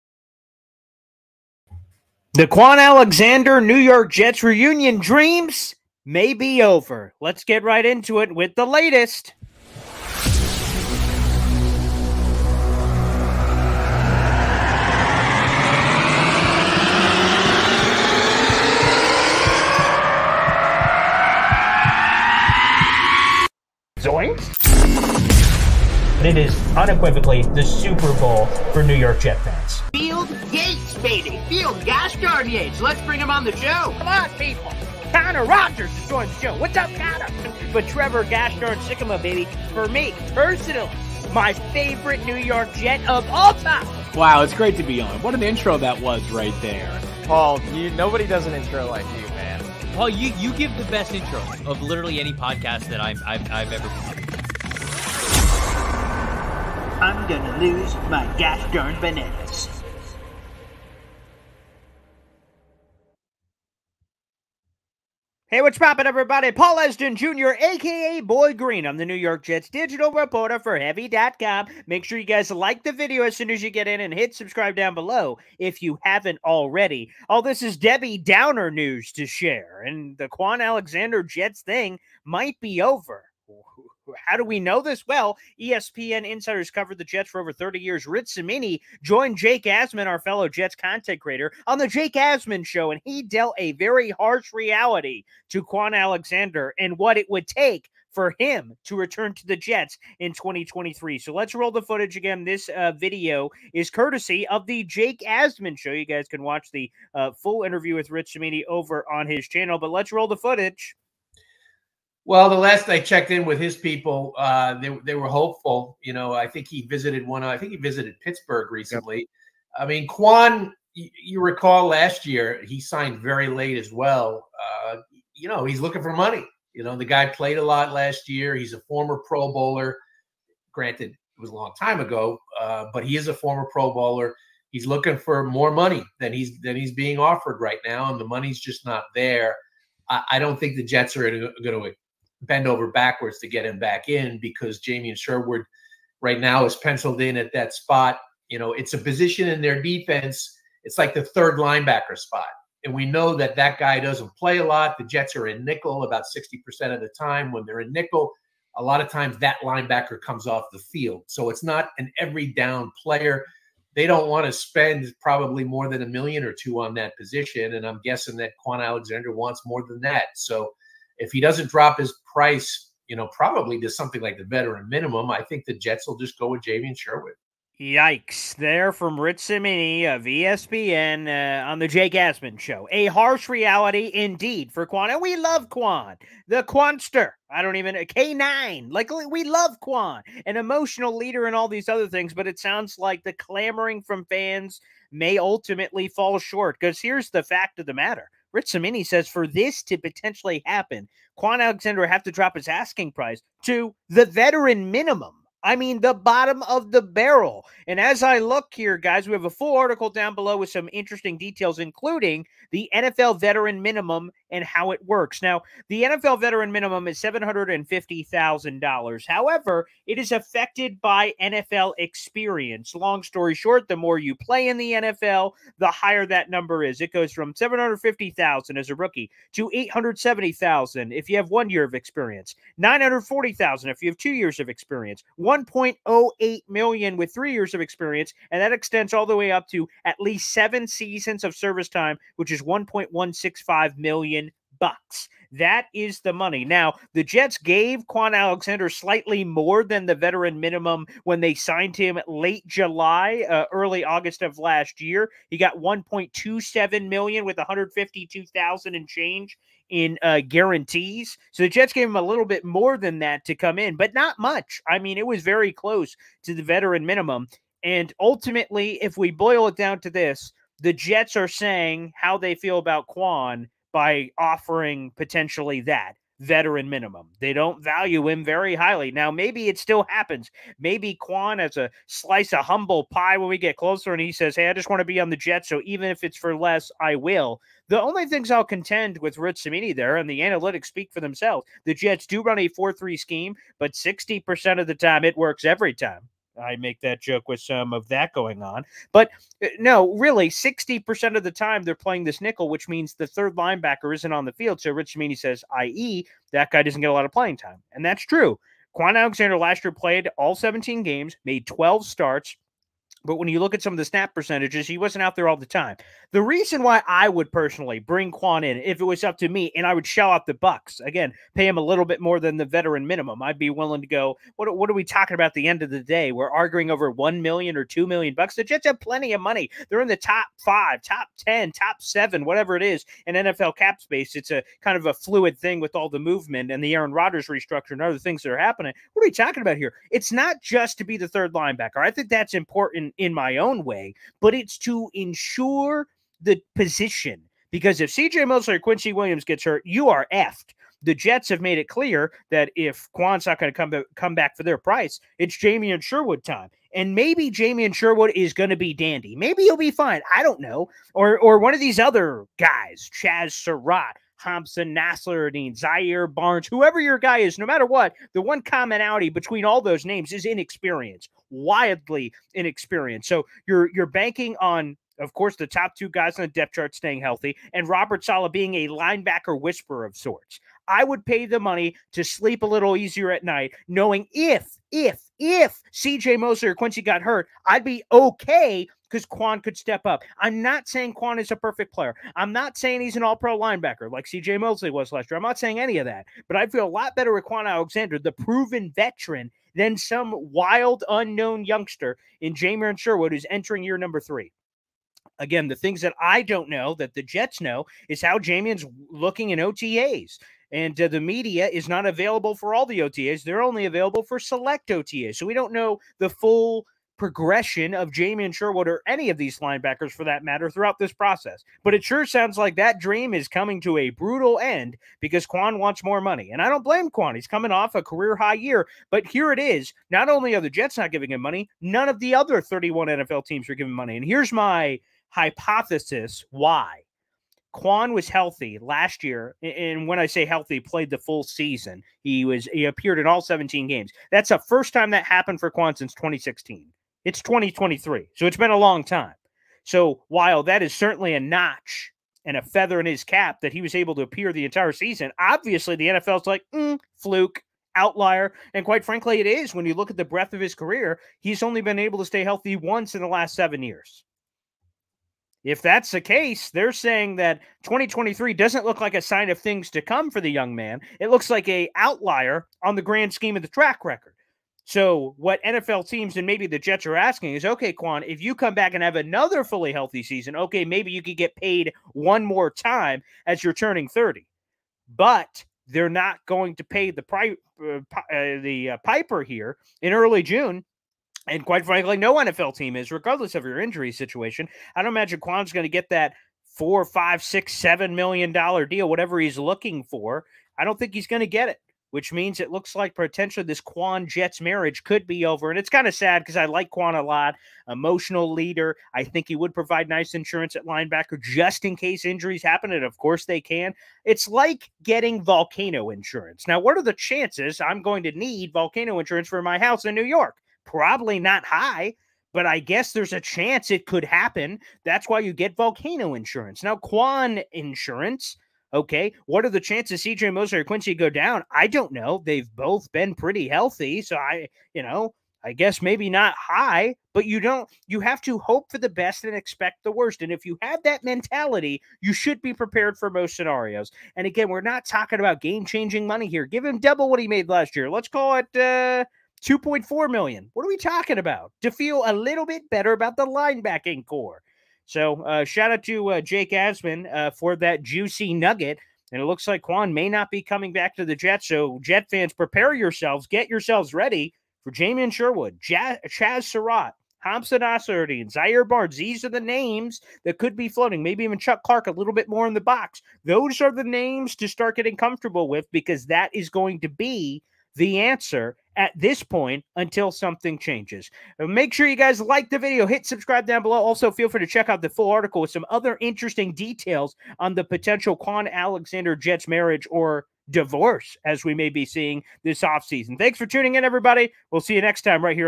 Ex Jets LB Kwon Alexander Gets HARSH Reality Check Amid Rumors
Boy Green reacts to the HARSH news with former Jets LB Kwon Alexander...